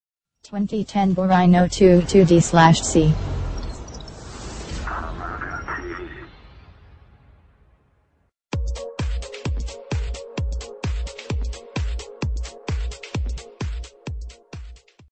2010 BAW RHINO 2.2 D/C
Full service history. All paper work in order. 2010 Baw Rhino 2.2 D/C is in showroom condition. Have the utmost confidence when buying this vehicle